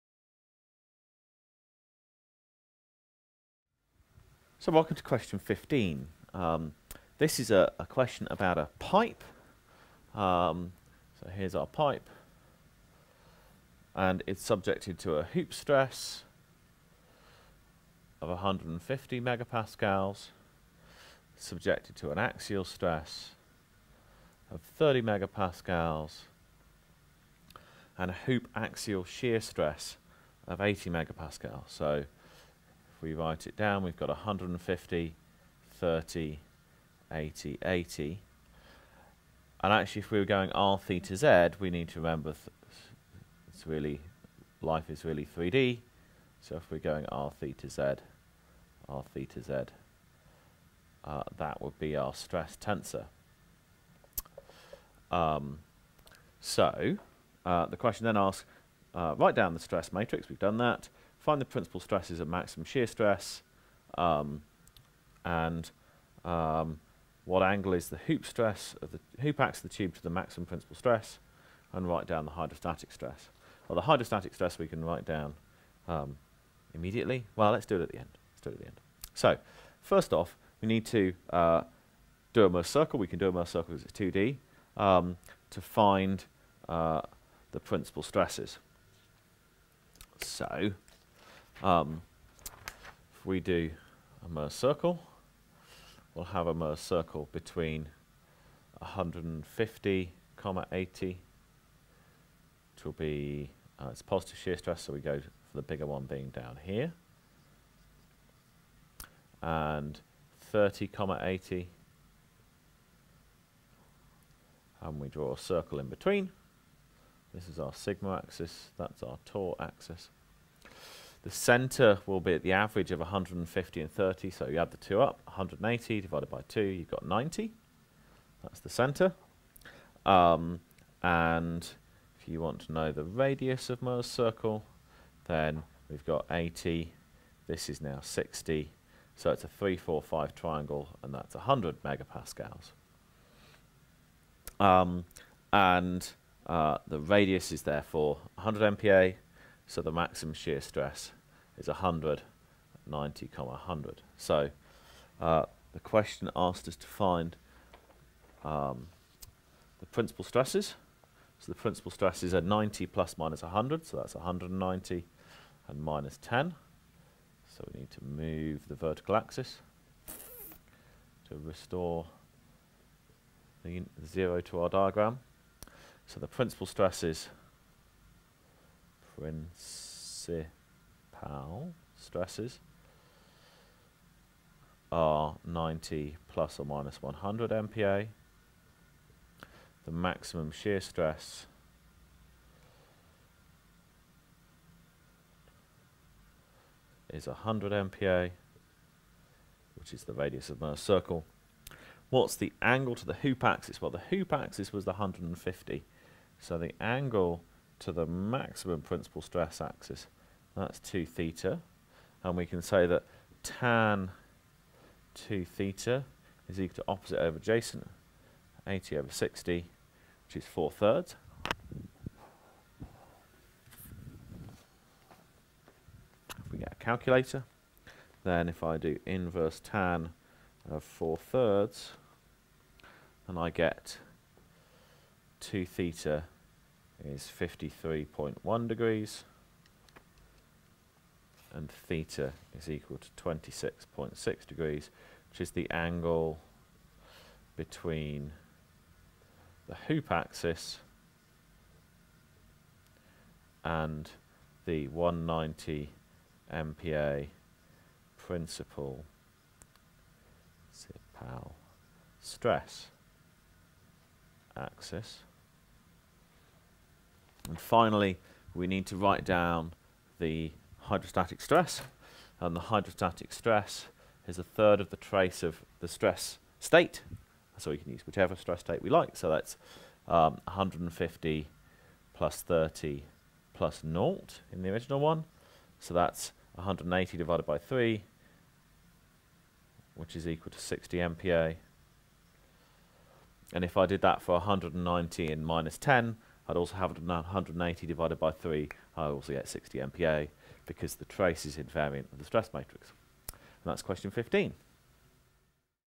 Q15 | MSE203 - A Mohr's circle on a pipe
Segment 3 of lecture 6. Q15 - Mohr's circle on a pipe
Lecturer: Dr David Dye.
Department of Materials, Imperial College, London, UK